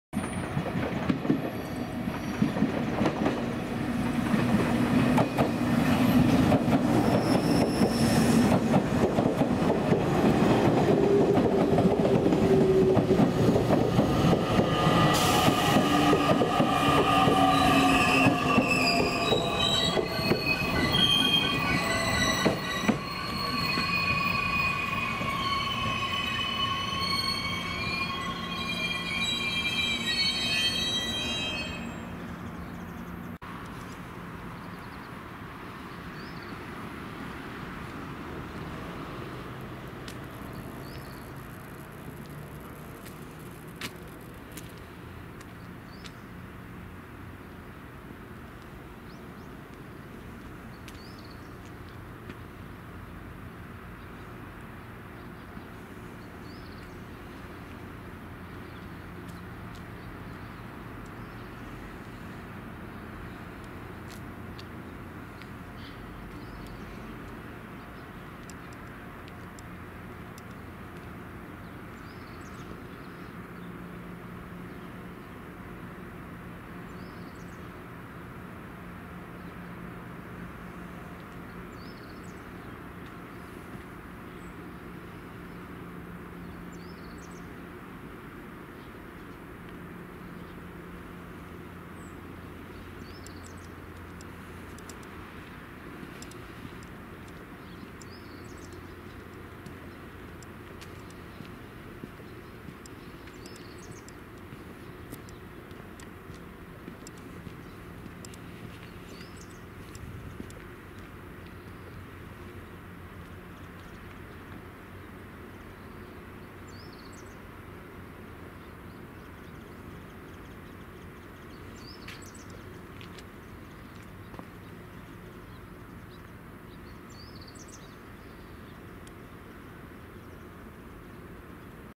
JR九州日豊本線 宇島駅／JR Kyushu Nippo Main Line
This is JR Kyushu Nippo Honsen Line Ushima Station taken on January 8, 2023.

2023年1月8日に撮影したJR九州日豊本線 宇島駅です。

Everyday Update!